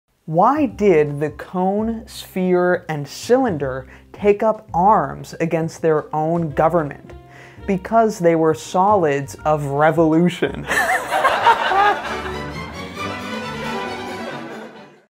government overthrowing solids joke
because cones, spheres, and cylinders are solids created by revolving a plane figure about an axis, and are thus called solids of revolution, and revolution means not only to to revolve but also to revolt, like revolt against your government, so it's funny   haha